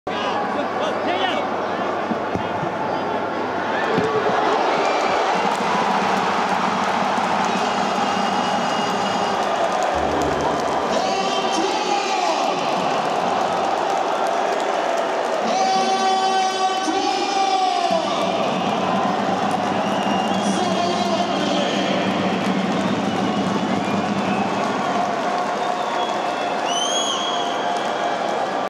Anton Zabolotny's Bicycle Goal against FC Rostov | RPL 2020/21
An amazing goal was scored tonight by Anton Zabolotny! Enjoy this bicycle kick that he made in a game against FC Rostov. He was named a Man of the Match.